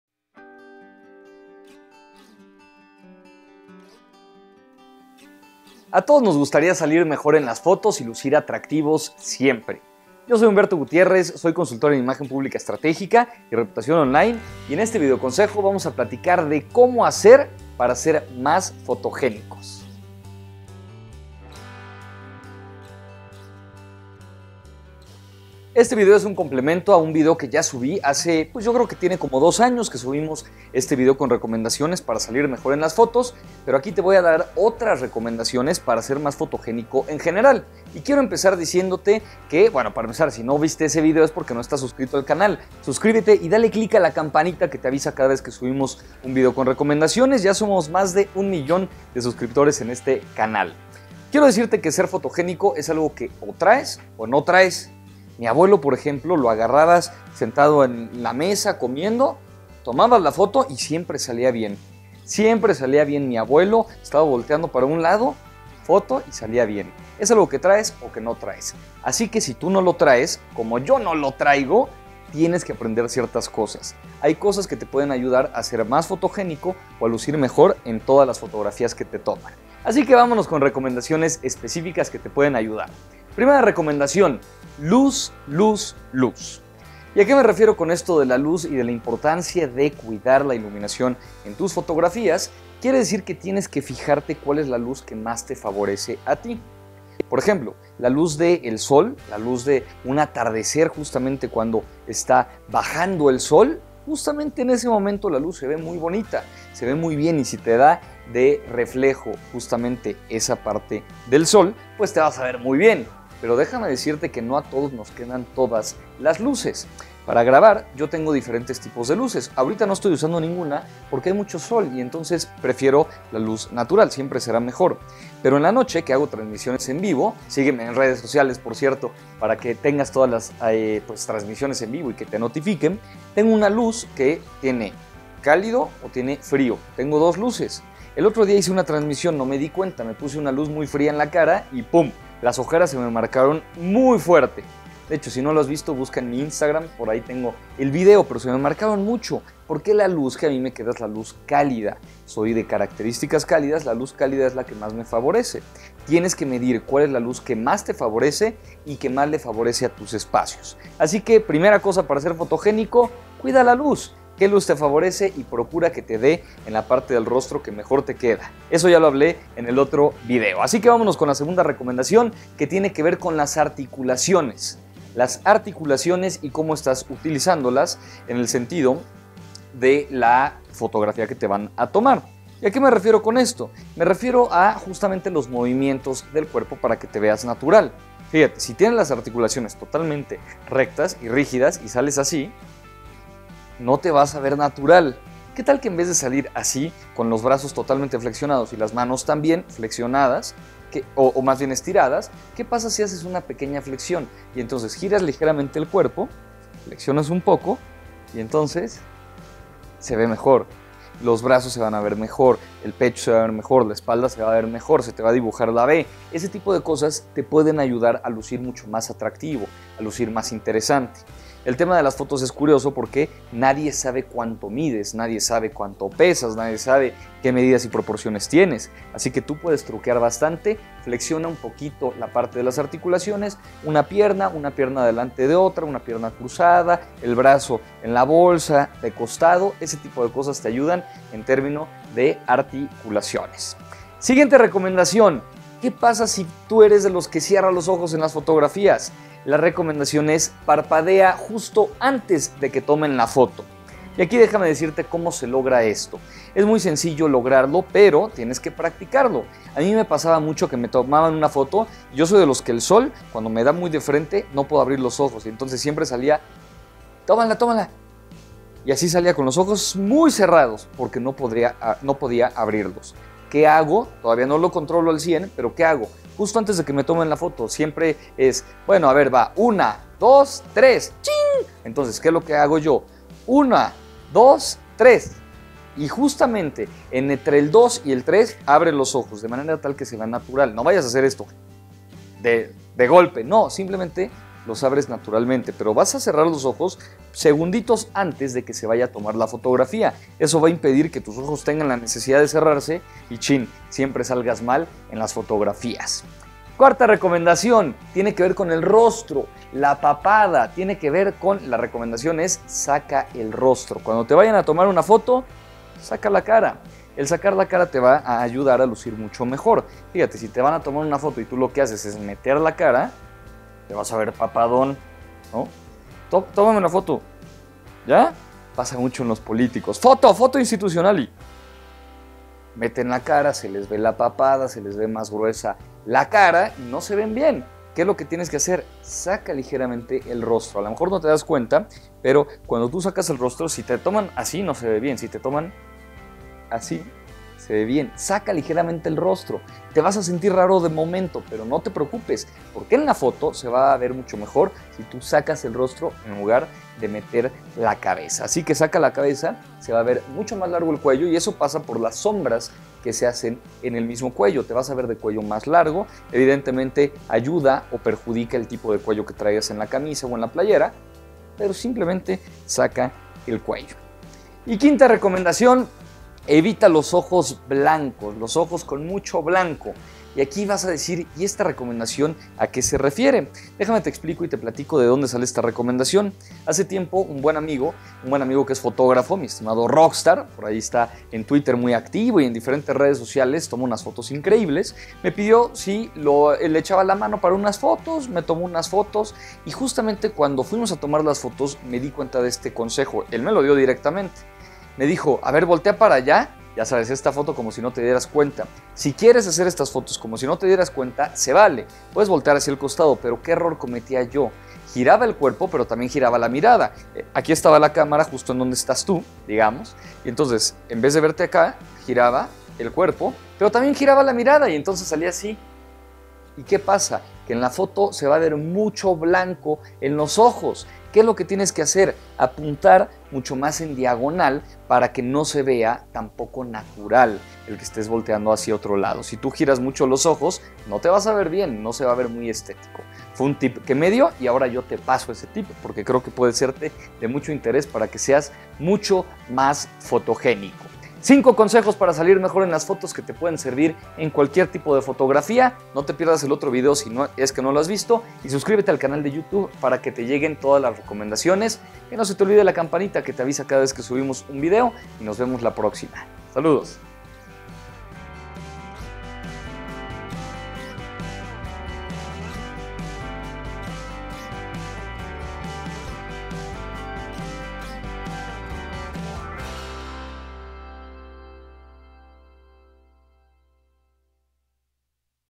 Cómo ser más FOTOGÉNICO - Cómo verte SEXY en cualquier FOTO
¿Te gustaría salir bien en cualquier foto? Es posible mejorar tus poses y la forma en la que te ves en cada foto siempre y cuando pienses en algunos elementos como la luz que tienes en el rostro, la forma en la que colocas la cara y el cuerpo y hasta la posición en la que tienes los ojos.

¡Sigue estos consejos, tips y recomendaciones para mejorar la forma en la que sales en tus fotos! Es momento de lucir sexy y bien siempre.